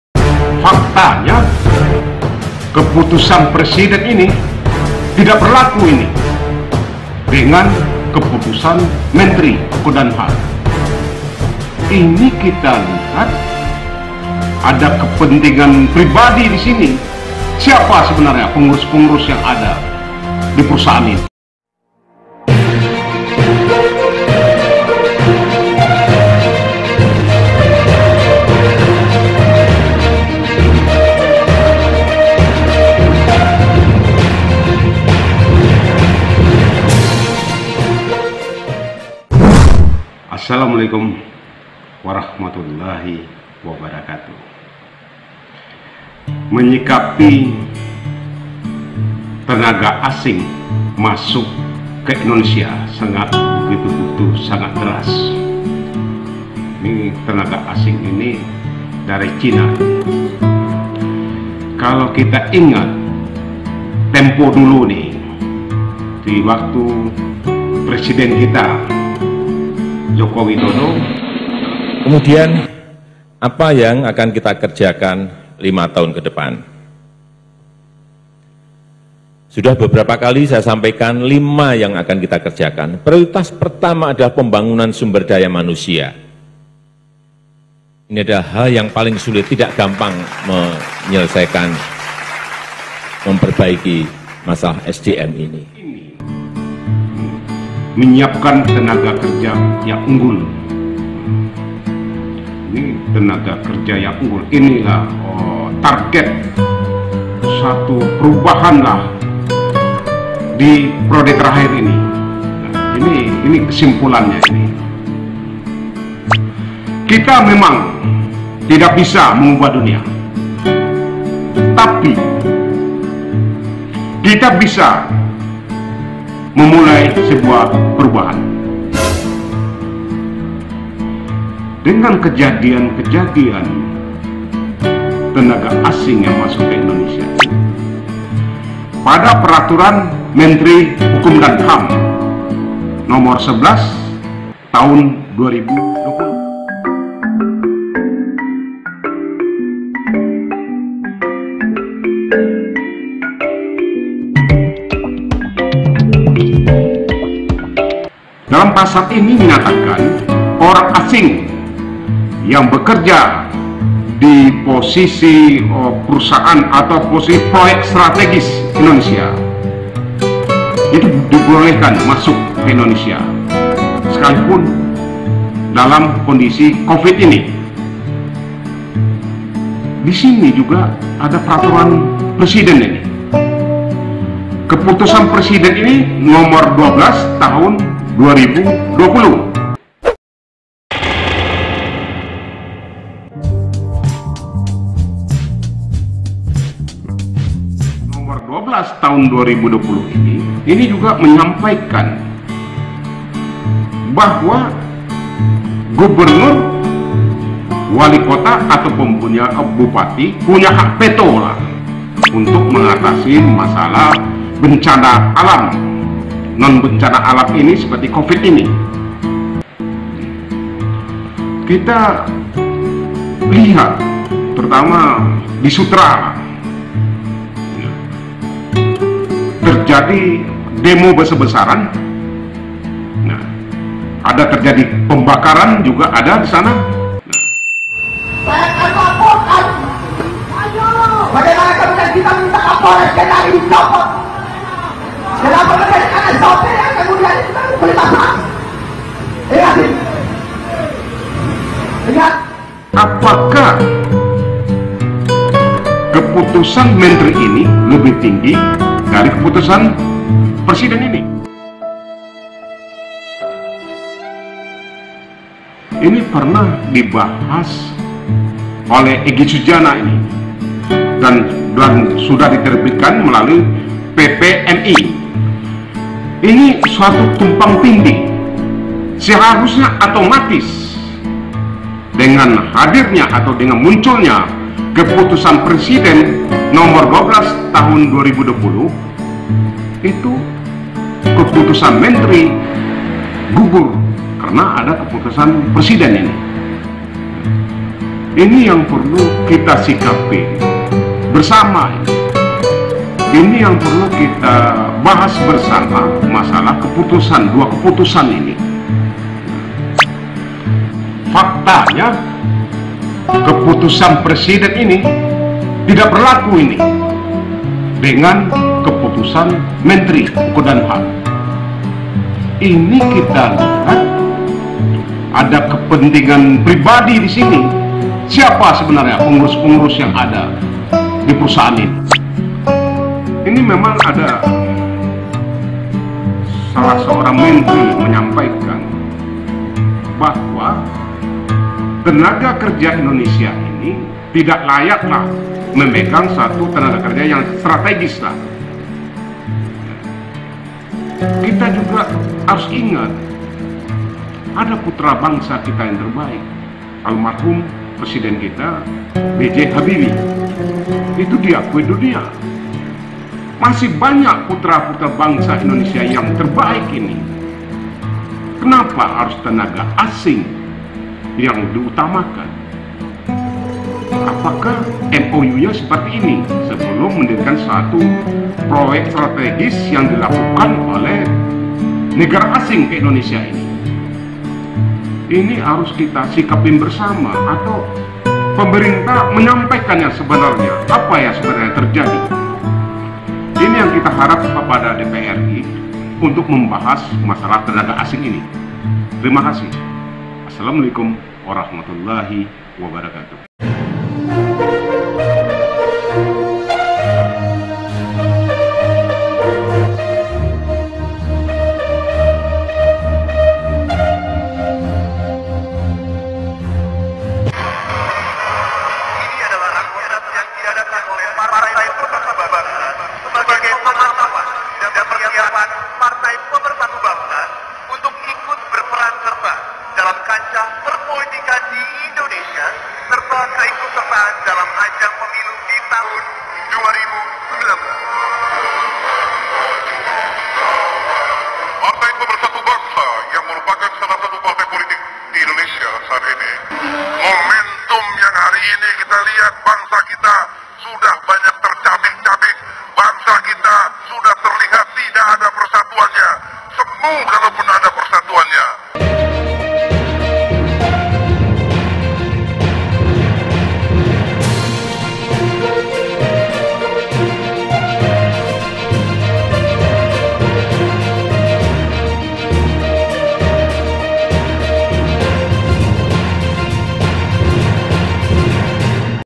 Keppres No.12 Tahun 2020 Daerah Berhak Tolak TKA CHINA di Tengah Bencana Non Alam
Kedatangan TKA China Harus Di lawan